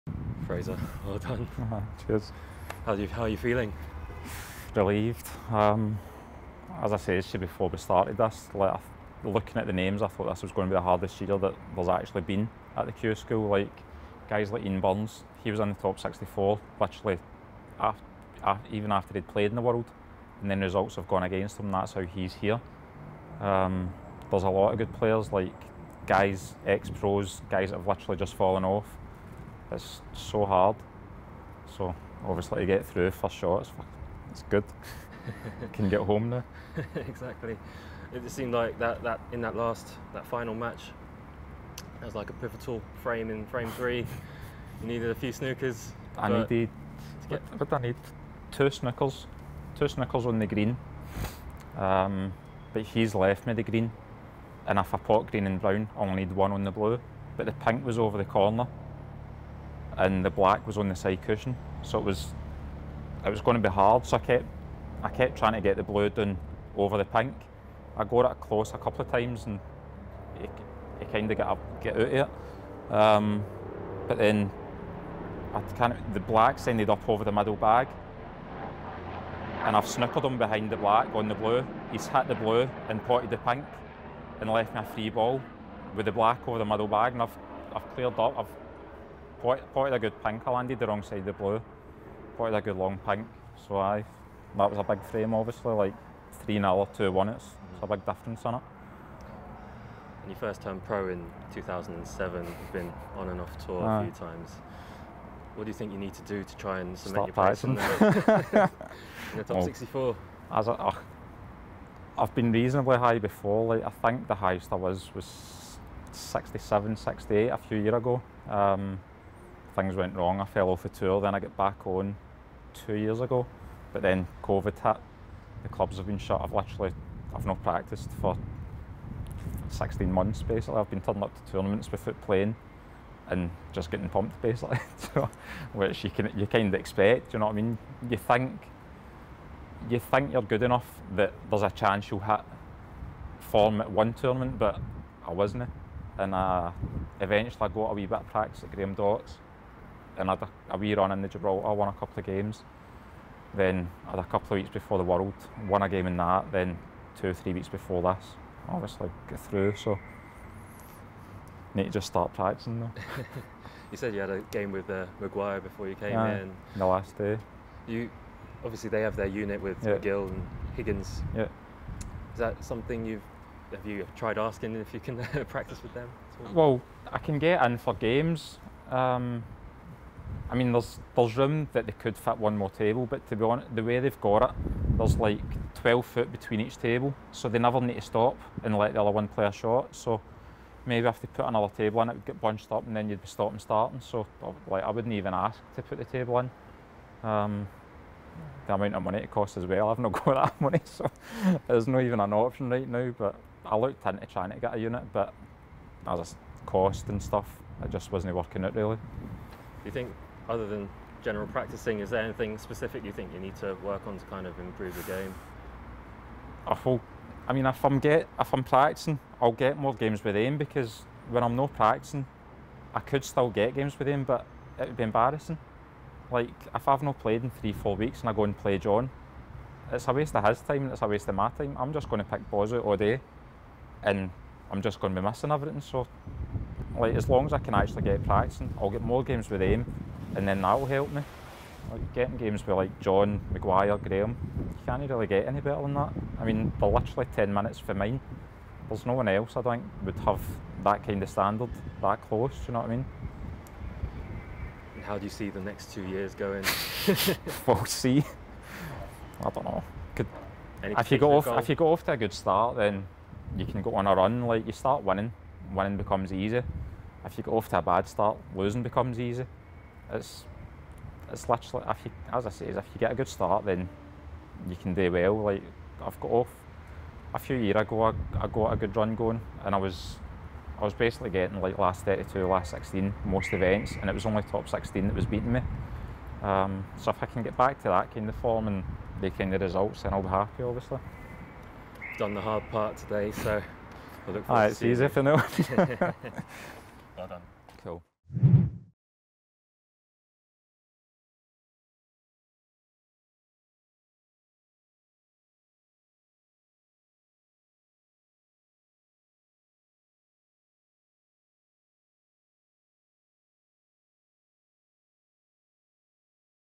Q School 2021 | Event 1 | Fraser PATRICK Earns Two-Year Tour Card At Q School
After receiving a fresh two-year tour card at Q School, Glaswegian cueist Fraser Patrick is hoping to make a fast start next season to kick on up the rankings.